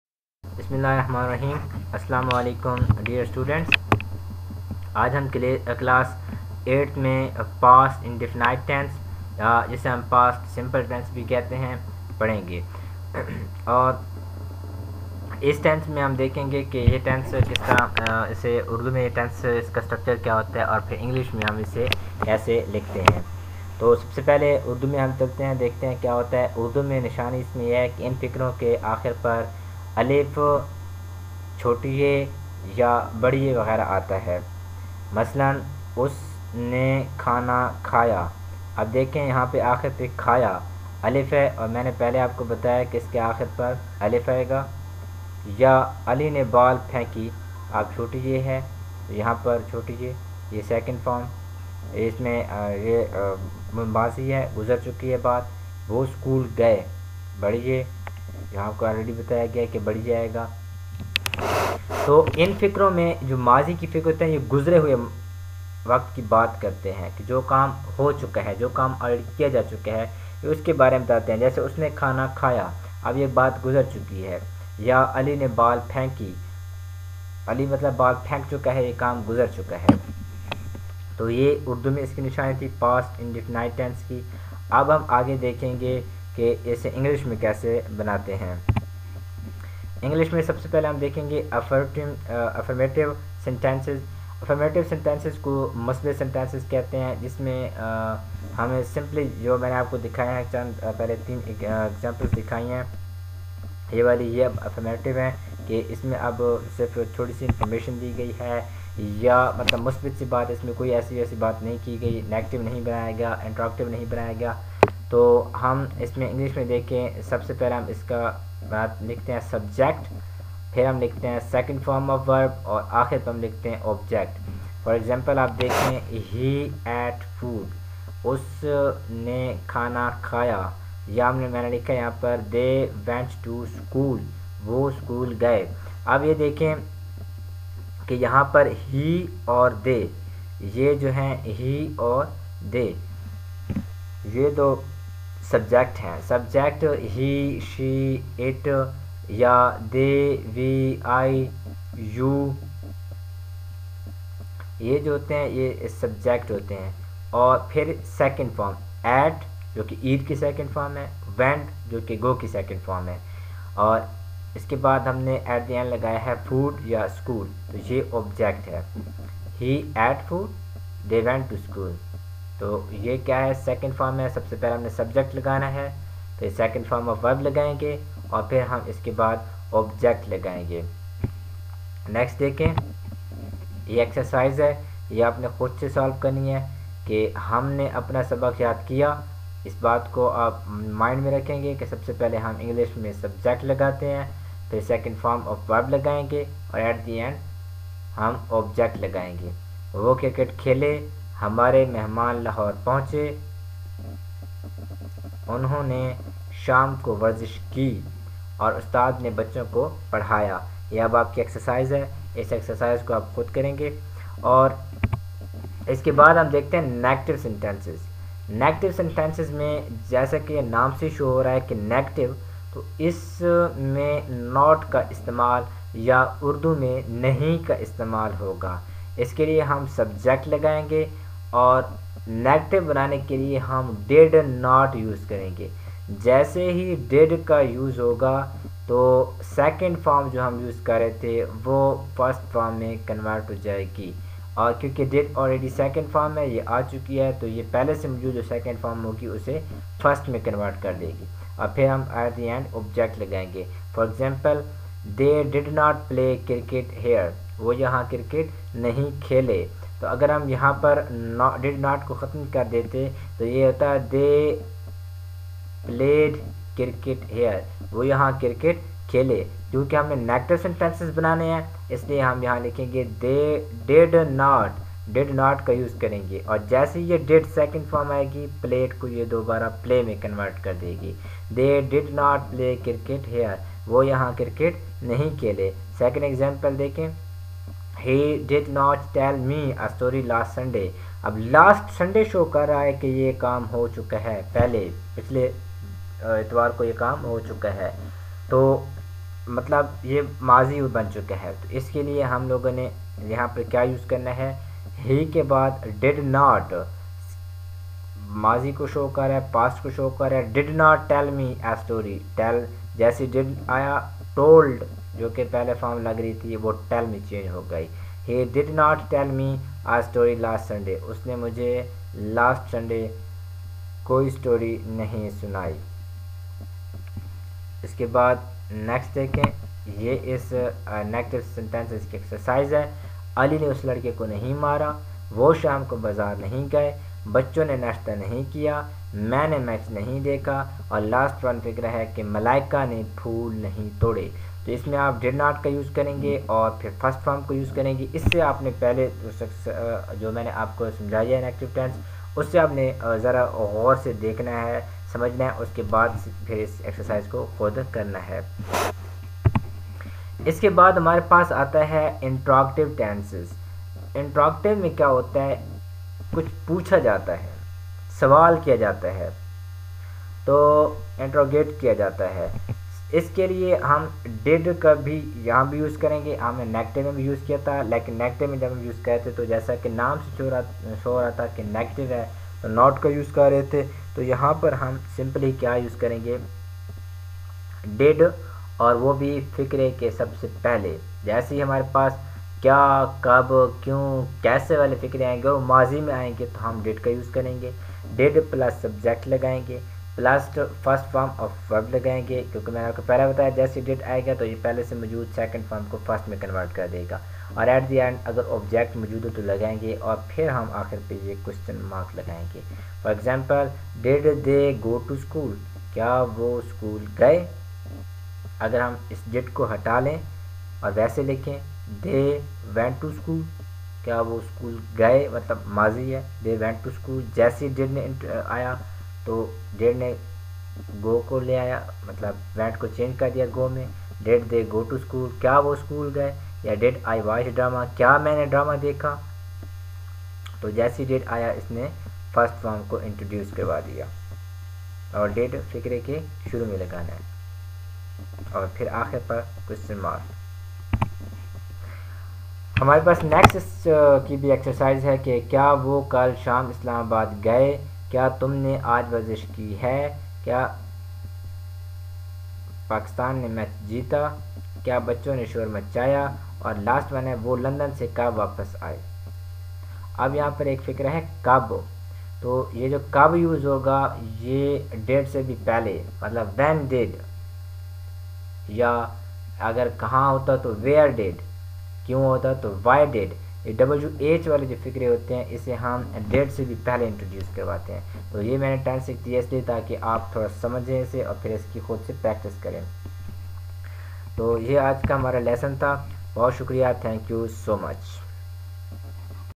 Class 8: Subject: English: Lecture No. 1
Class 8, Subject: English: Lecture 1: Past Indefinite Tens.